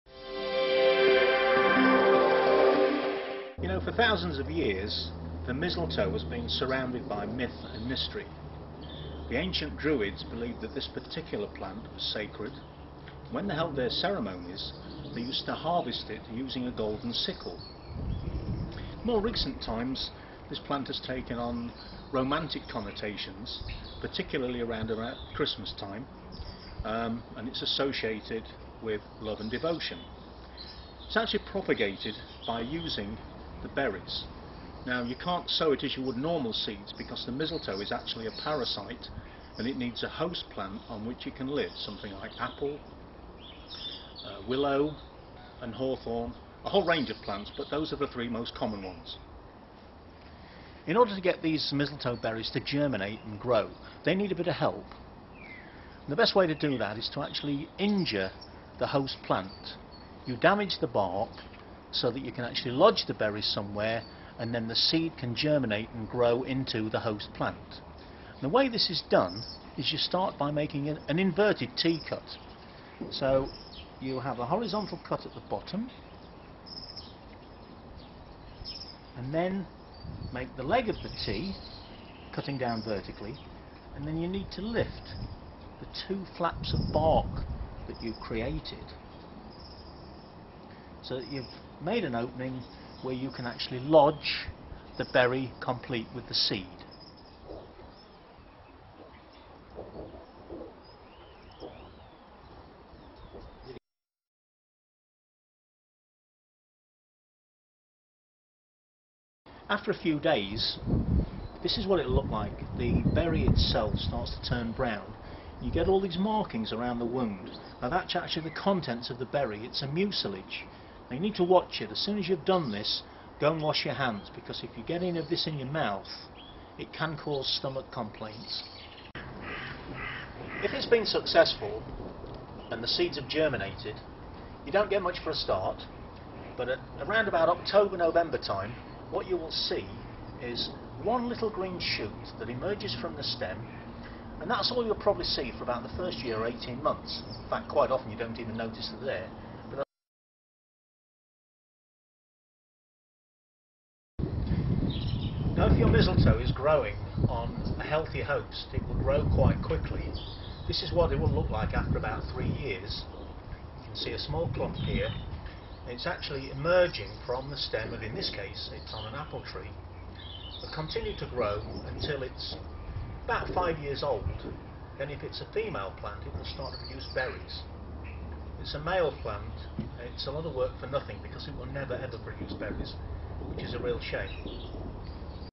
Grafting mistletoe
How to propagate mistletoe by grafting it to a host plant.